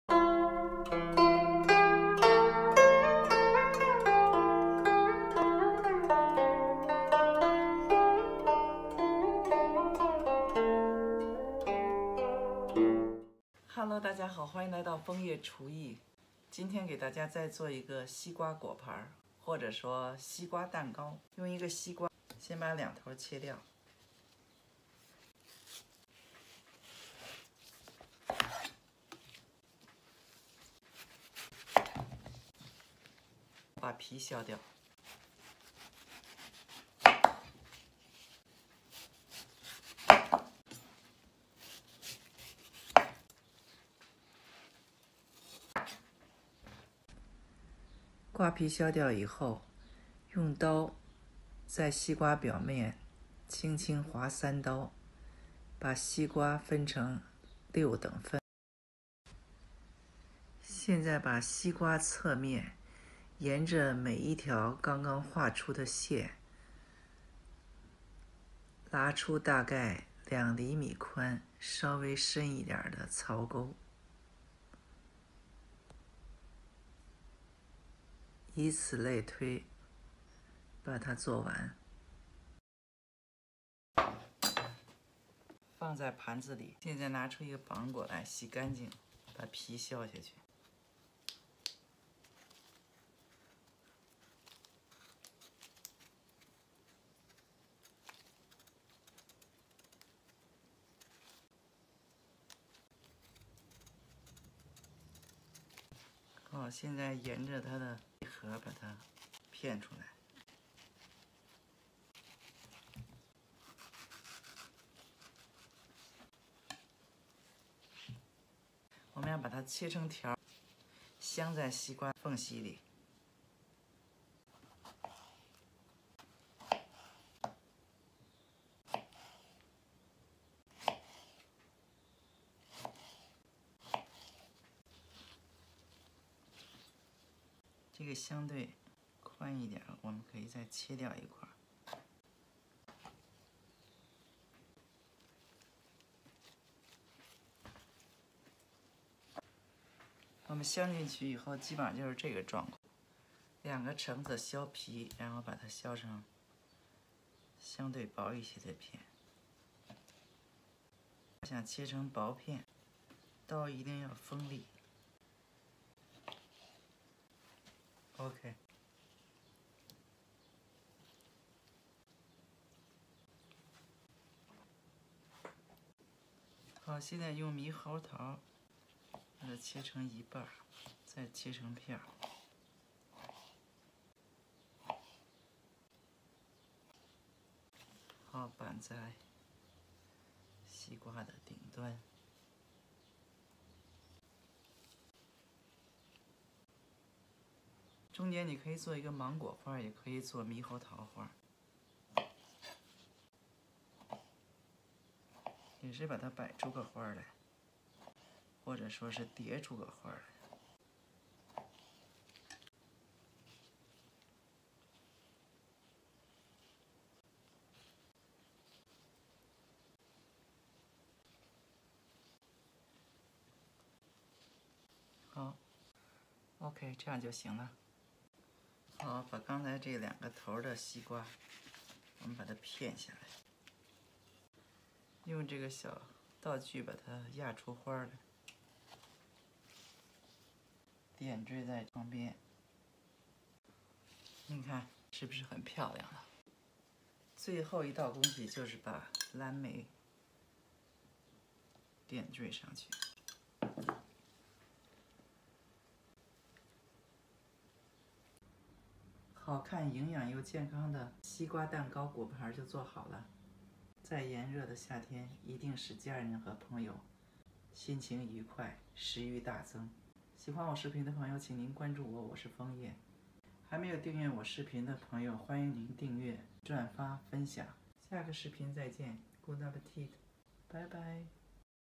111 Wassermelonen-Kuchenkompott 西瓜蛋糕果盘
西瓜蛋糕果盘
1个西瓜，1个芒果，2个橙子，2个猕猴桃，100克蓝莓。

Wassermelonen-Kuchenkompott Zutaten: 1 Wassermelone, 1 Mango, 2 Orangen, 2 Kiwis, 100g Blaubeeren.

Watermelon Cake Compote
Ingredients: 1 watermelon, 1 mango, 2 oranges, 2 kiwis, 100g of blueberries.

请您观看我的其他视频